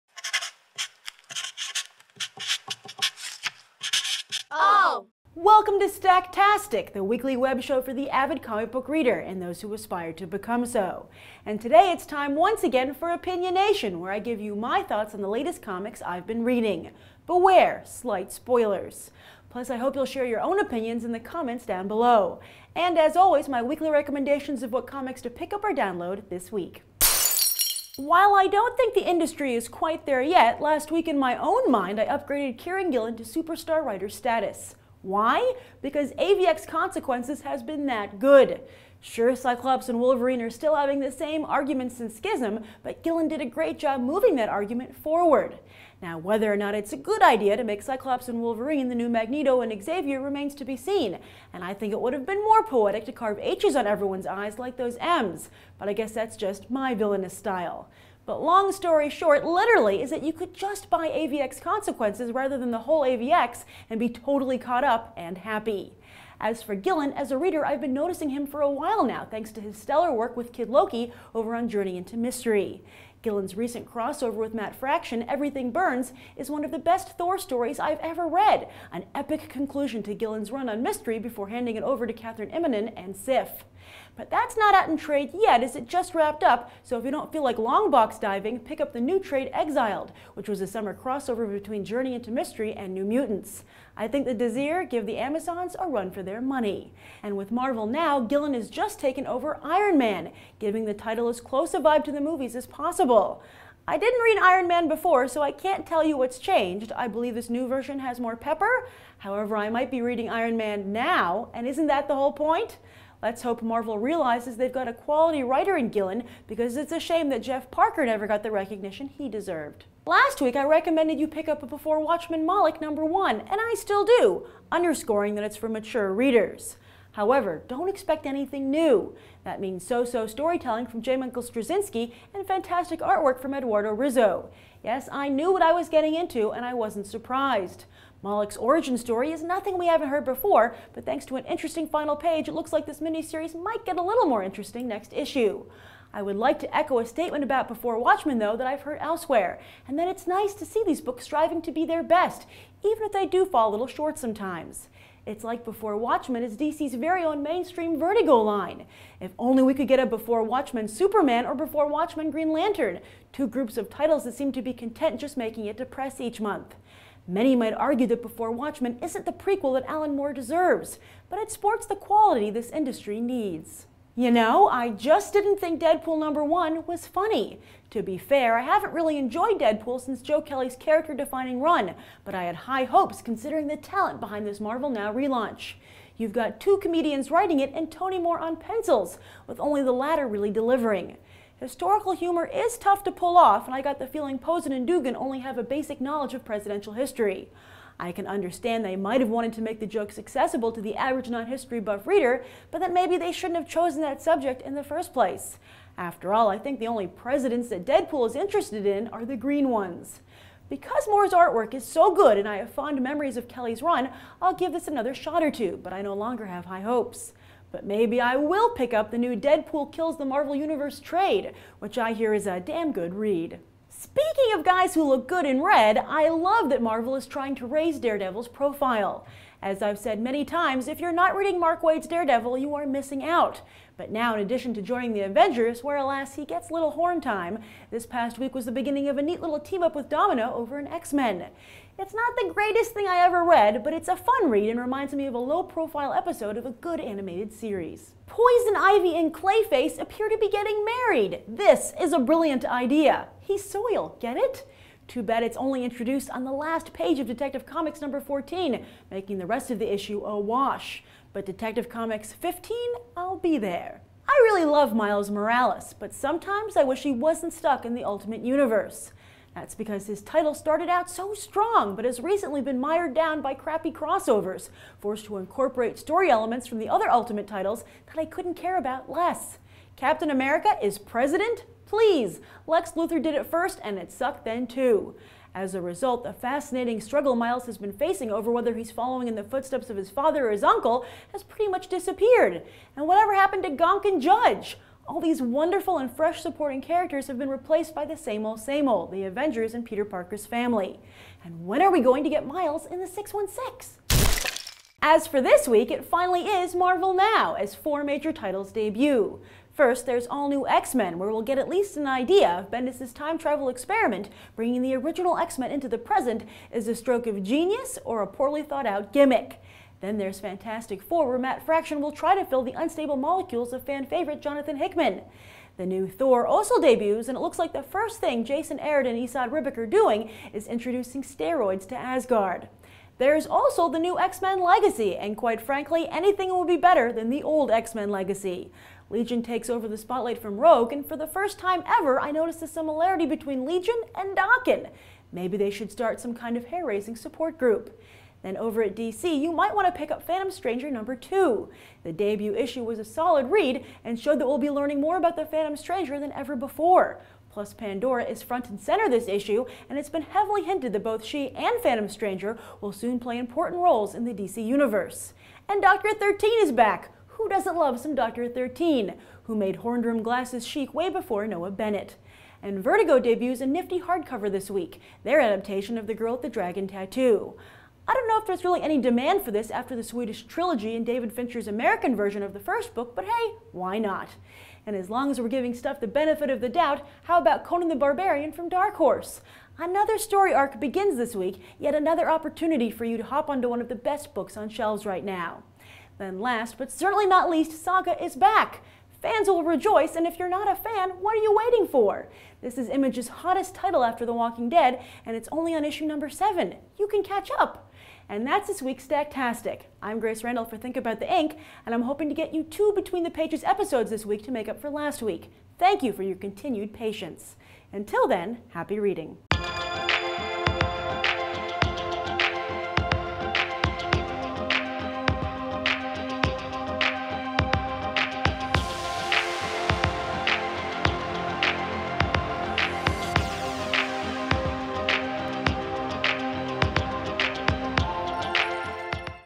Marvel Now Review Iron Man #1 and Deadpool #1 + Miles Morales, Before Watchmen Moloch!
The Marvel Now relaunch is beginning!  This week host Grace Randolph gives her review of Iron Man #1 and Deadpool #1!  Plus also on Opinion Nation, her thoughts on why Miles Morales shouldn't be stuck in the Ultimate Universe, and why Before Watchmen Moloch is a sign of the good Before Watchmen does for the comic book industry!  Are you enjoying Marvel Now?  Be sure to share your own review of Iron Man #1 and Deadpool #1 in the comments section!  Plus recommendations of what DC, Marvel, Dark Horse, Image and other comics to buy this week!  hink About The Ink is presented by Bleeding Cool.